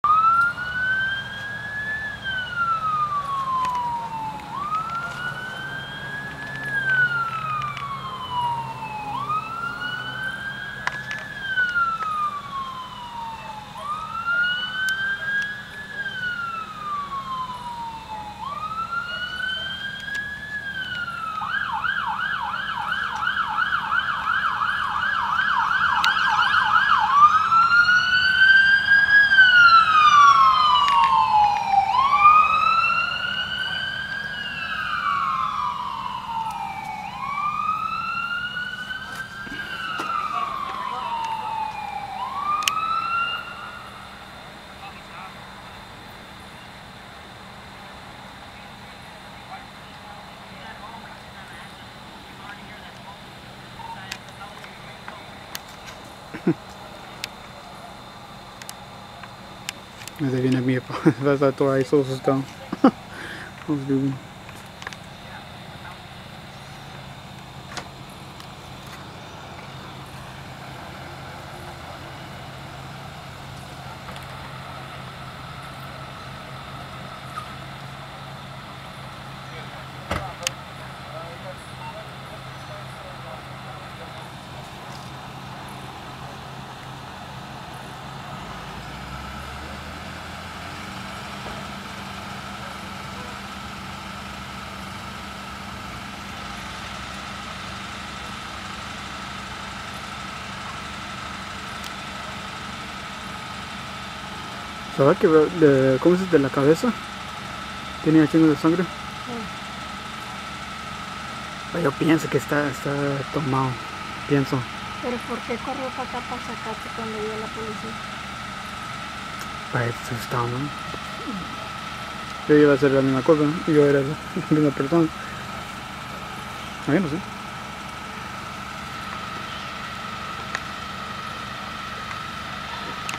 Guy crashed into my lightpost and mail box...Whatever!!
Well sorry haven't been bringing new vids to my channel but this happen today or now i guess some kid crashed into my post then taking down mail box & destroyed the water hydrant but the guys ok helped him but just was bleeding from his head but again wanted to take video but didn't want the cops to tell me shit sorry if short.